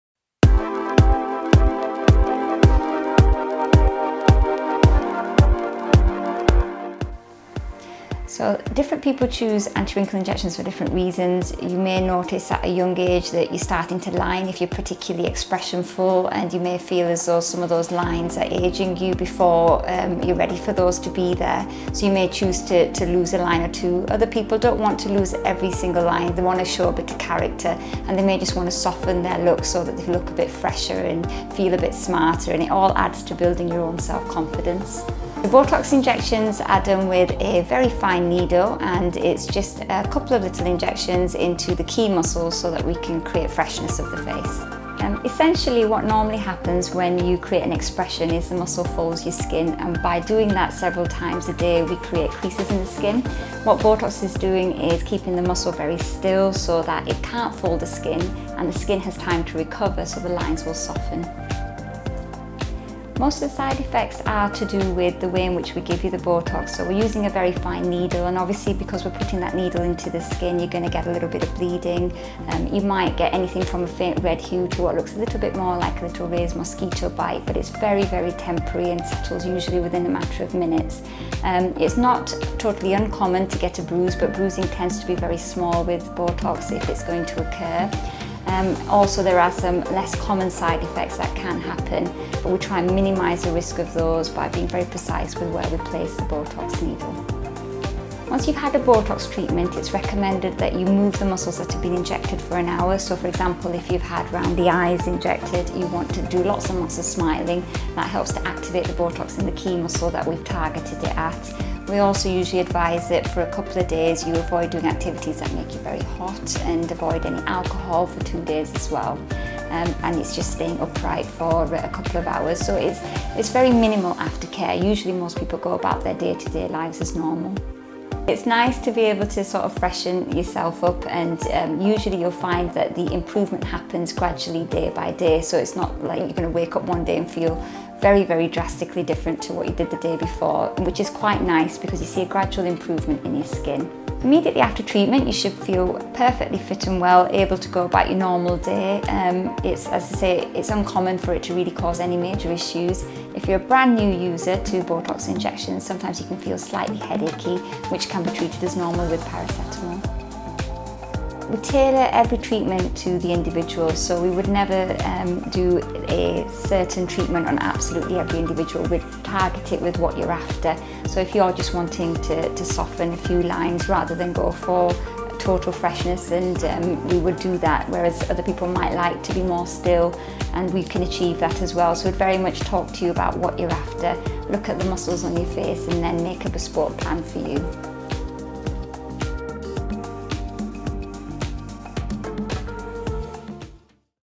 Can men have anti-ageing treatment? | Explanation by Dr. Sharan | SkinViva Manchester
SkinViva's Dr Sharan, gives us the low down on anti-ageing treatments for men alongside a live action video of a real treatment on a male client.